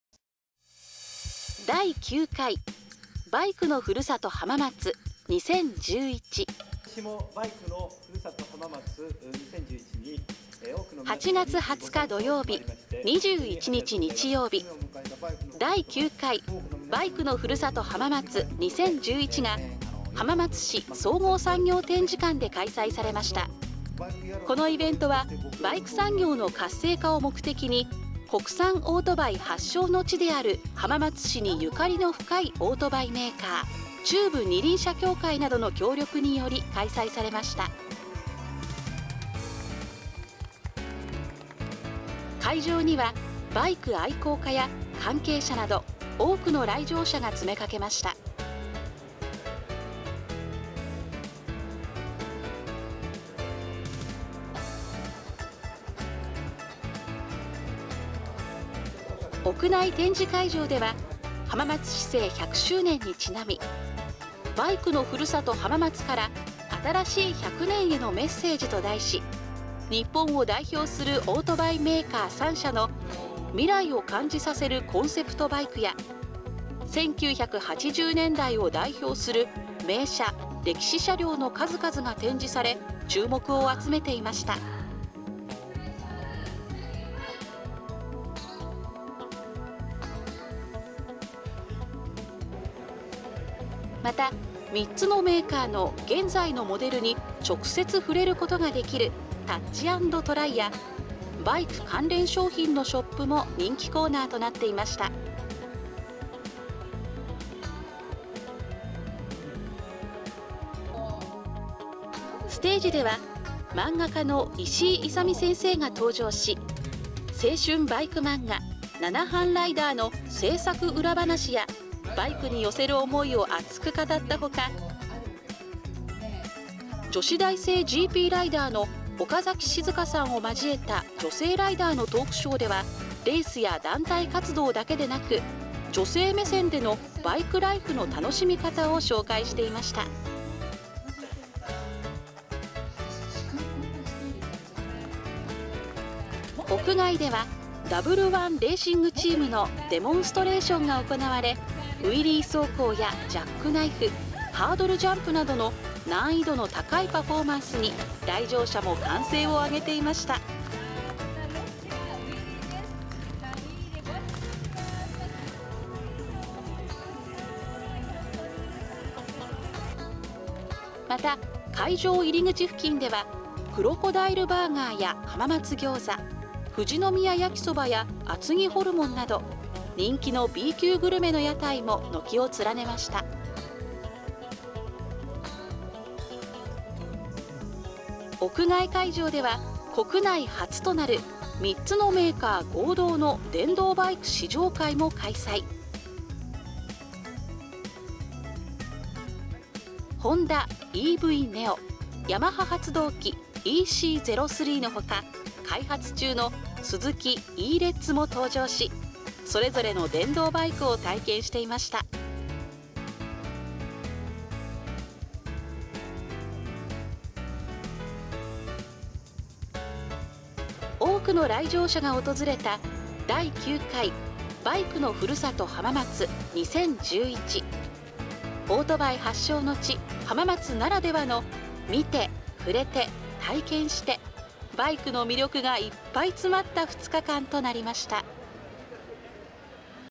第9回 バイクのふるさと浜松2011
見て、触れて、体験してバイクの魅力いっぱいの2日間になりました。(平成23年9月)